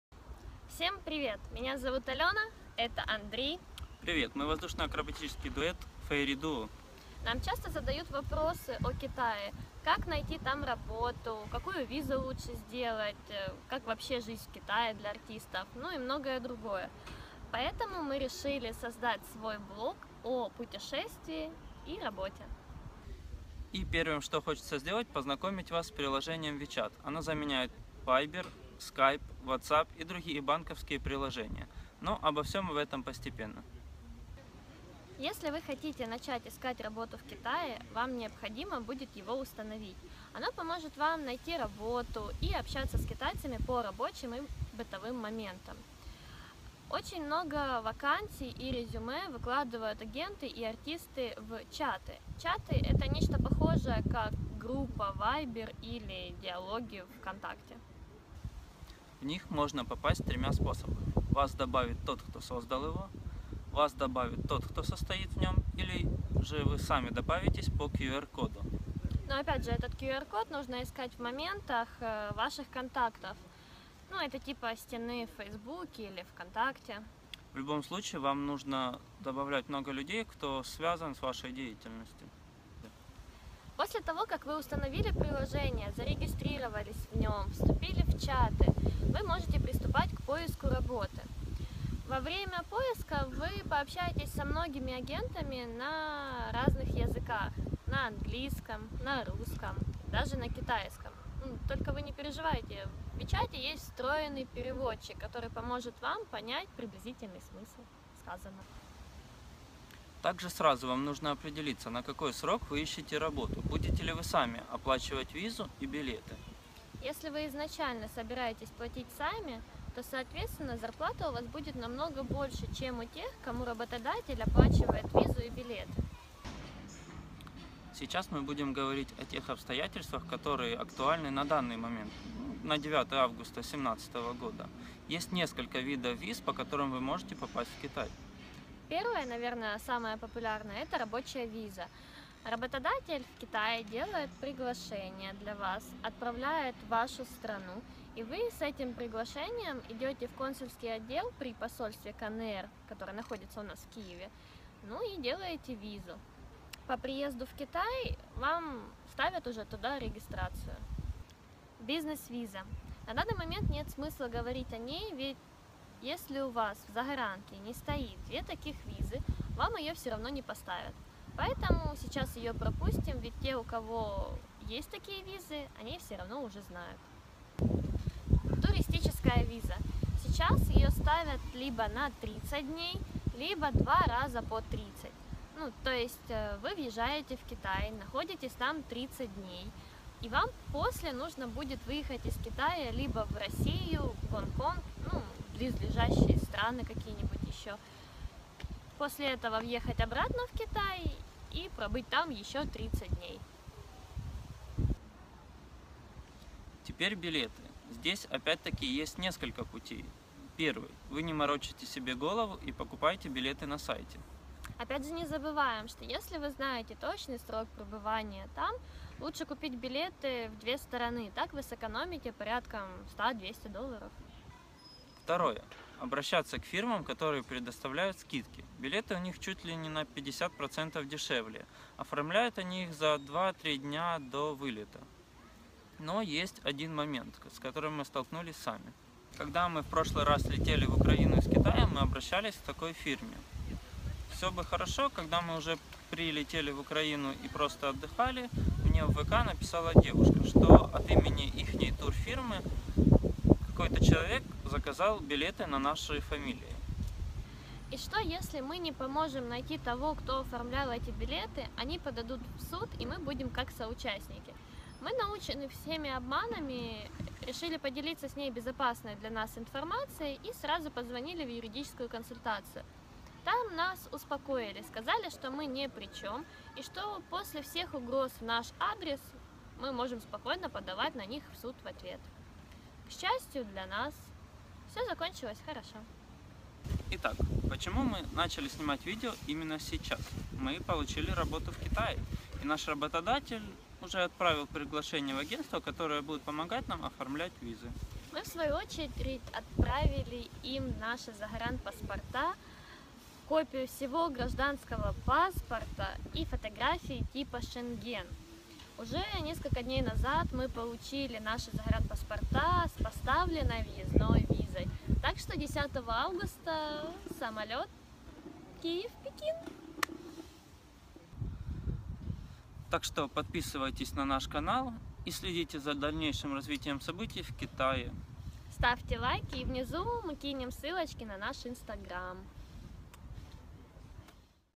Fairy duo. Блог. Китай для артистов. 1 выпуск
В этом выпуске мы расскажем:
1. Как найти работу в Китае. Приложение wechat
2. Виза
3. Билеты

Этот выпуск информационный. Мы решили сконцентрировать ваше внимание на важные вопросы при поиске работы в Китае.